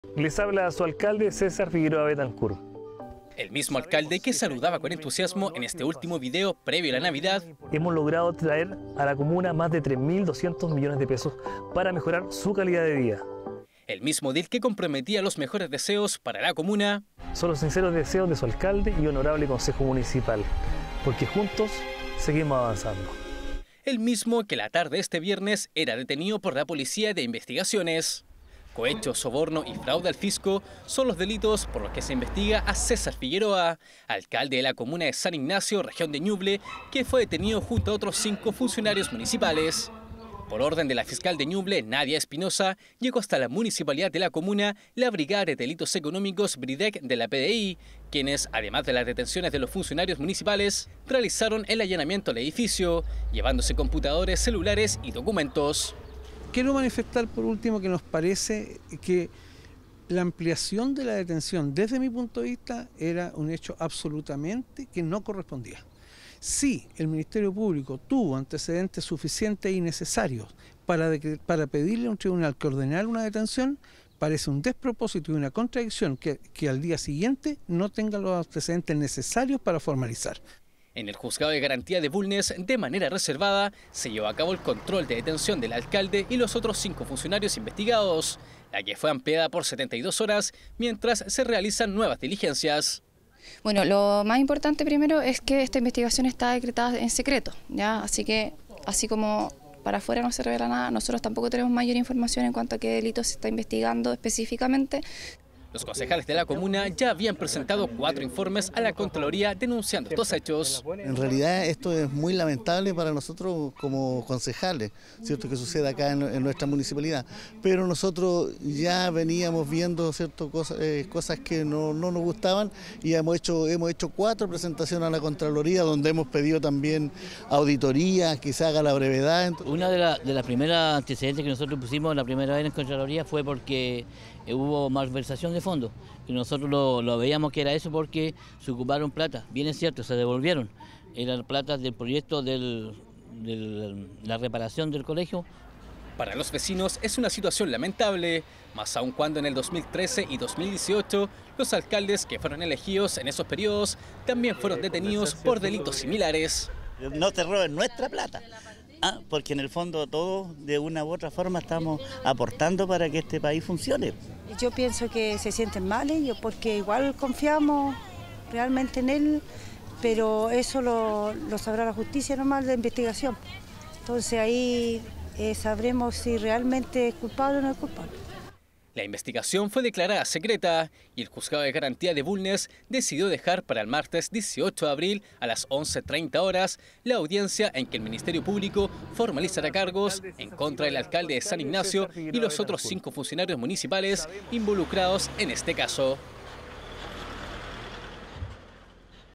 Detienen a alcalde por cohecho y malversación
El alcalde de la comuna de San Ignacio, Cesar Figueroa, en la región de Ñuble, fue detenido junto a 5 funcionarios municipales por eventual cohecho, malversación de fondos y fraude al fisco.